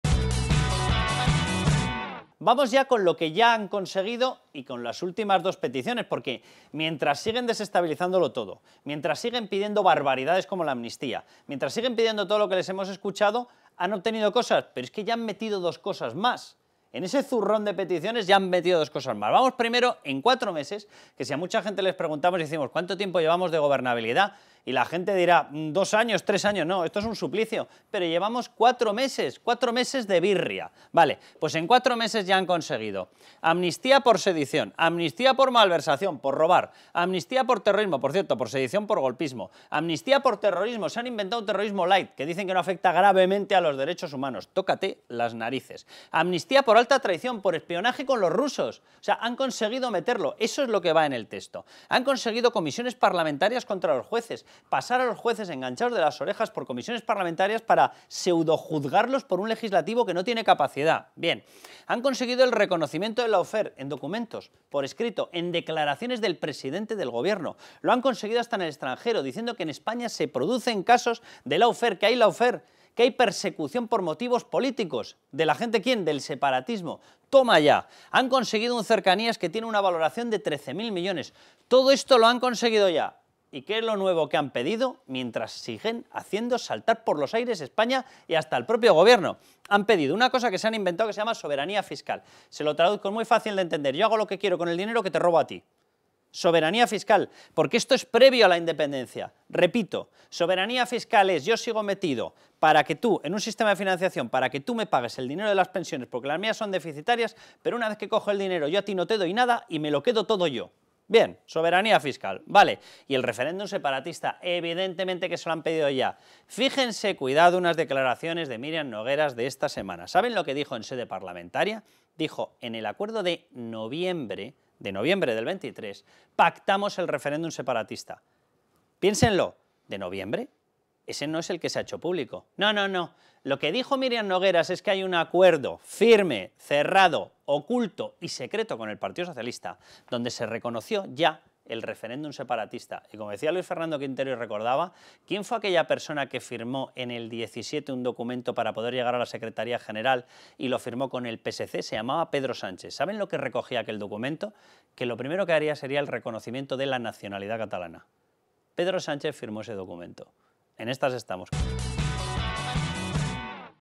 Este es el siguiente paso de los separatistas y Puigdemont
Carlos Cuesta explica el siguiente paso de los separatistas con Puigdemont a la cabeza.